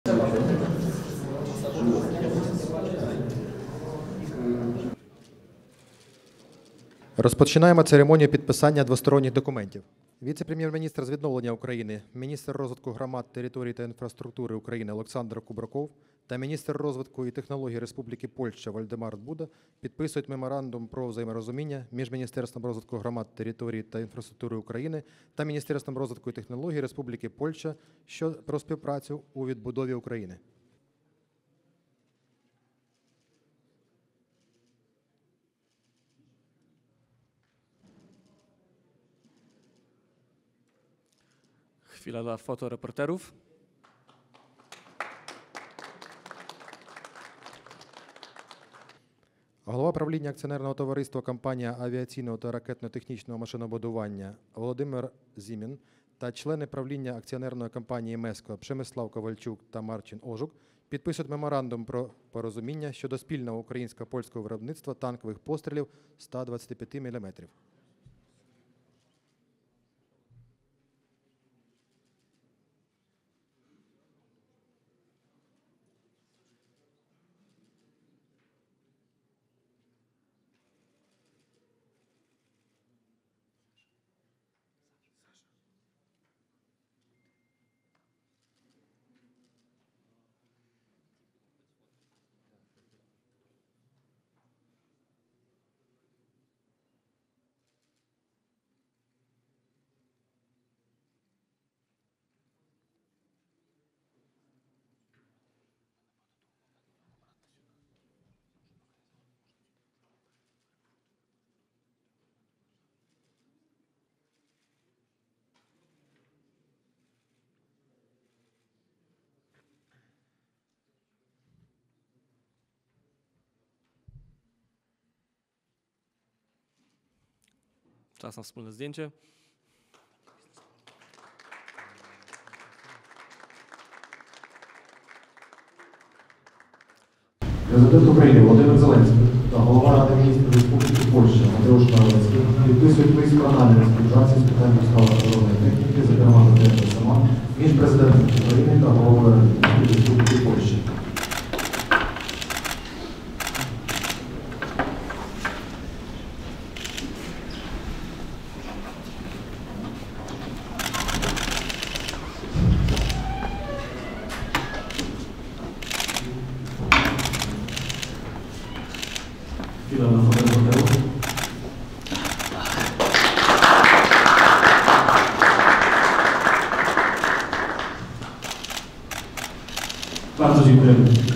Україна та Польща підписали Лист про наміри співпраці з питань постачання оборонної техніки
Метою підписання Листа є забезпечення постійної підтримки та зміцнення оборонних потужностей ЗСУ шляхом придбання Україною польської оборонної техніки, зокрема БТР «Росомак», самохідних мінометів «Рак» та ракет до переносних зенітно-ракетних комплексів «Перун».

Також у межах візиту Президента до Республіки Польща підписано Меморандум про взаєморозуміння між Міністерством розвитку громад, територій та інфраструктури України та Міністерством економічного розвитку й технологій Республіки Польща про співпрацю у відбудові України.

Впевнений, що наше відновлення допомагатиме розвитку і польського бізнесу, і обох наших країн», – сказав Глава держави.